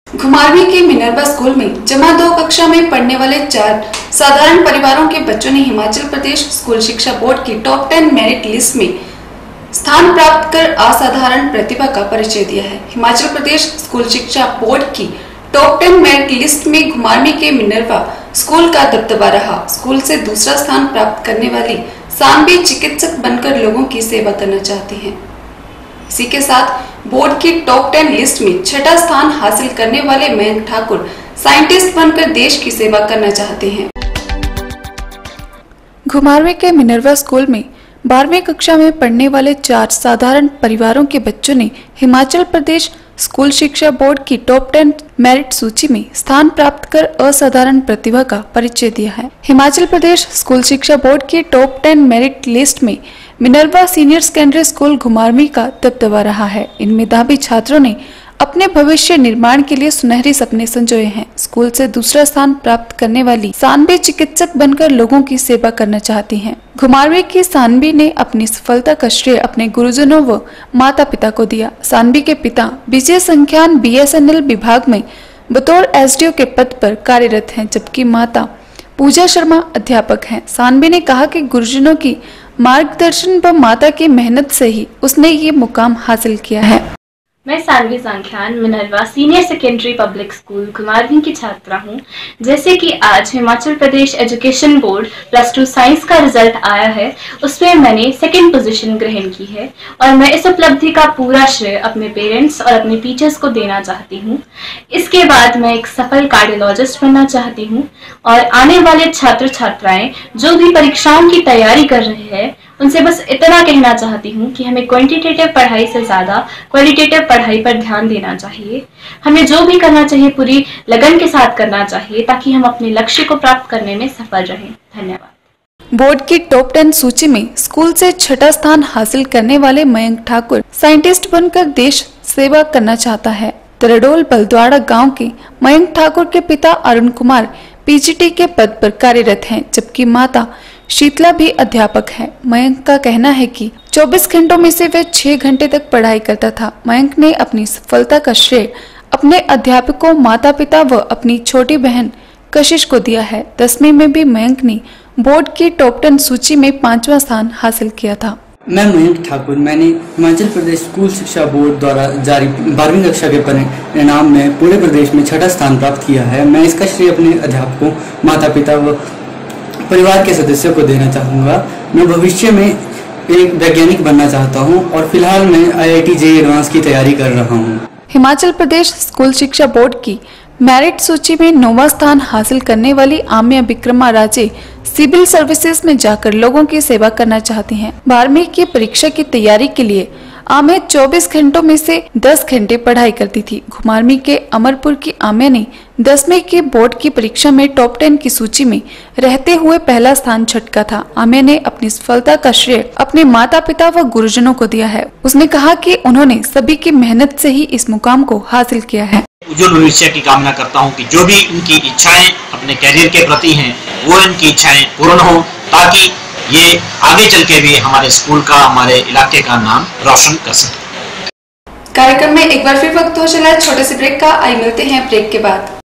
23 N 7 B2 At the top ten merit list of the H. P. Board, Minerva School is dominated by
घुमारवीं के मिनर्वा स्कूल में जमा दो कक्षा में पढ़ने वाले चार  साधारण परिवारों के बच्चों ने  हिमाचल प्रदेश स्कूल शिक्षा बोर्ड की टॉप टेन मैरिट लिस्ट  में स्थान प्राप्त कर असाधारण प्रतिभा का परिचय दिया है हिमाचल प्रदेश स्कूल शिक्षा बोर्ड की टॉप टेन मैरिट  लिस्ट  में घुमारवीं के मिनर्वा स्कूल का दबदबा रहा। स्कूल से दूसरा स्थान प्राप्त करने वाली सानवी चिकित्सक बनकर लोगों की सेवा करना चाहती है बोर्ड की टॉप टेन लिस्ट में छठा स्थान हासिल करने वाले मयंक ठाकुर साइंटिस्ट बनकर देश सेवा करना चाहता है। और बोर्ड की मैरिट लिस्ट में नोवां स्थान हासिल करने वाली आमया बिक्रमा राजे सिविल सर्वसिस में जाकर लोगो की सेवा करना चाहती है।